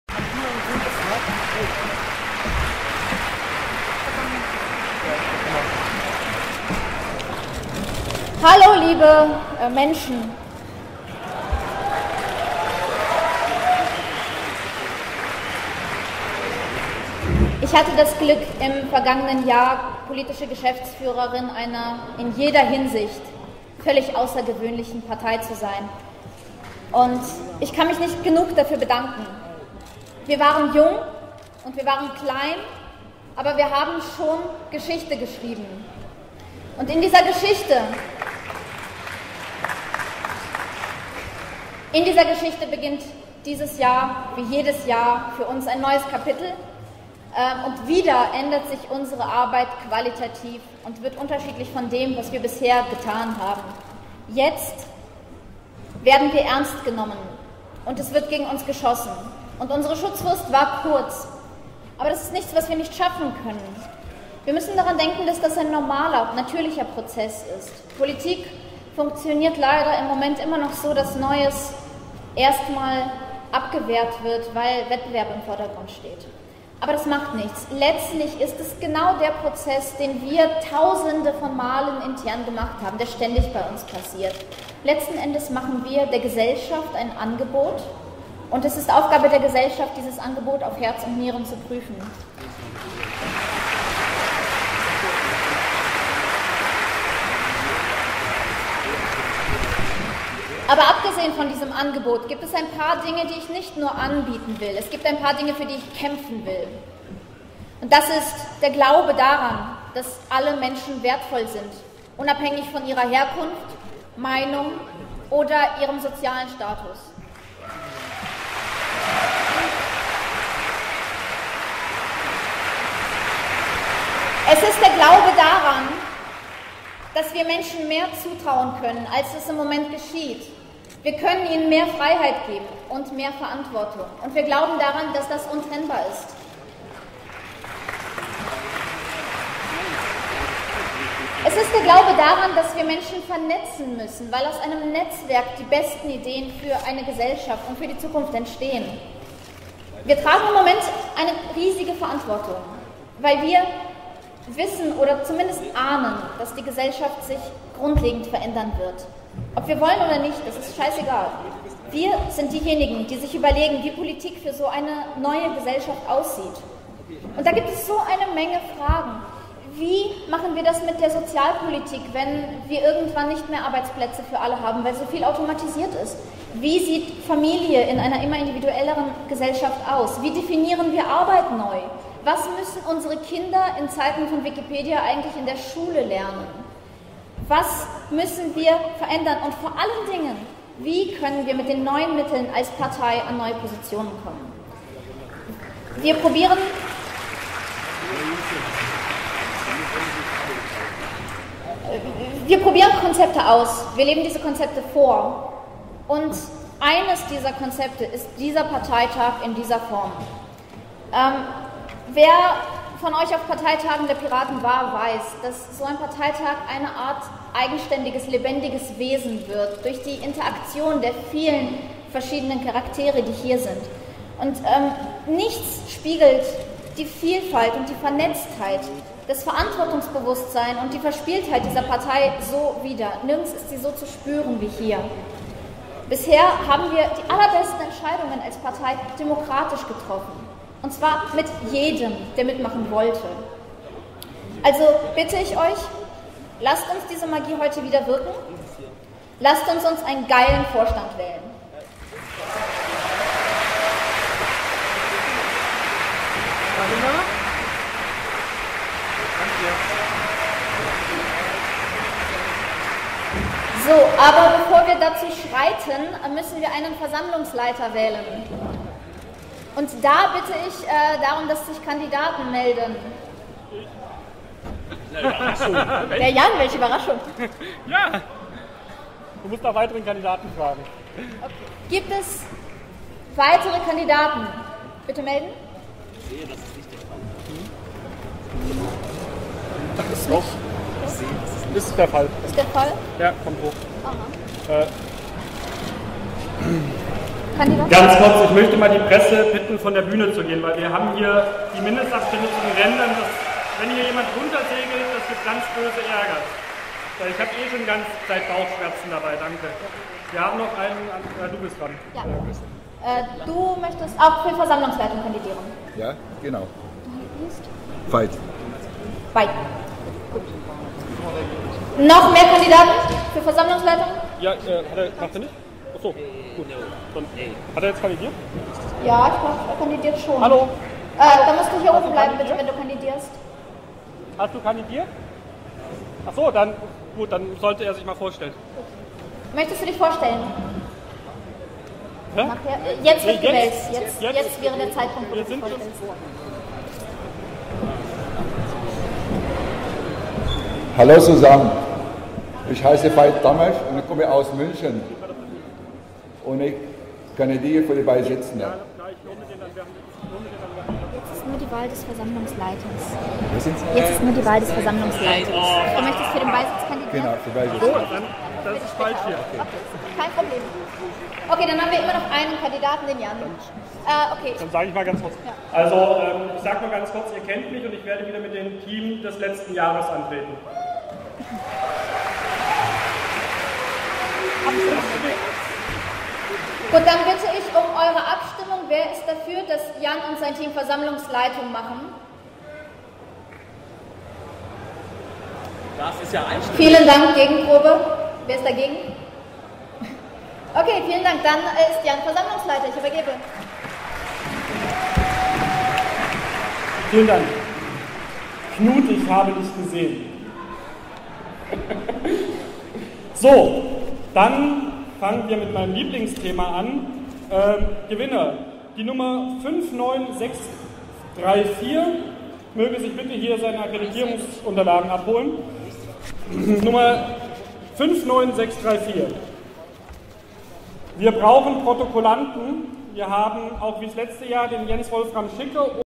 Marina Weisband eröffnet den Bundesparteitag der Piratenpartei 2012 in Neumünster
Marina eröffnet den Bundesparteitag der Piraten 2012 in Neumünster. Sie freut sich die Partei bis jetzt im Vorstand vertreten zu haben. Wegen ihrer Diplomarbeit steht sie nicht zur Wiederwahl. Sie ruft dazu auf einen geilen Vorstand zu wählen.

CC-BY Carsten Neumann